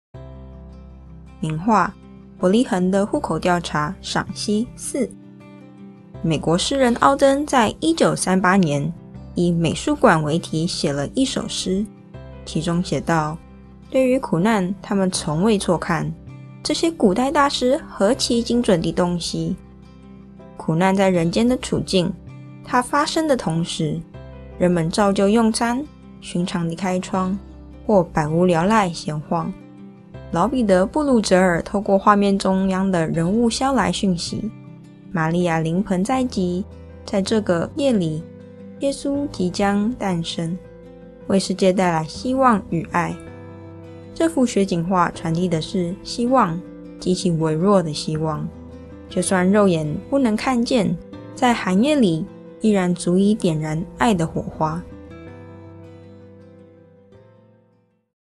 【名畫賞析】《伯利恆的戶口調查》The Census at Bethlehem 賞析四｜2019第四屆「食在有藝思 x 耶誕愛美食」聖誕市集｜輔仁大學外語學院
畫家：老彼得．布魯哲爾（Pieter Bruegel the Elder）
年代背景：1566年
畫作解說：美國詩人奧登在一九三八年以《美術館》為題寫了一首詩，其中寫道：
　　對於苦難他們從未錯看
　　這些古代大師何其精準地洞悉
　　苦難在人間的處境：它發生的同時
　　人們照舊用餐，尋常地開窗，或百無聊賴閒晃

老彼得．布魯哲爾透過畫面中央的人物捎來訊息：瑪利亞臨盆在即，在這個夜裡，耶穌即將誕生，為世界帶來希望與愛。這幅雪景畫傳遞的是「希望」。極其微弱的希望，就算肉眼不能看見，在寒夜裡，依然足以點燃愛的火花。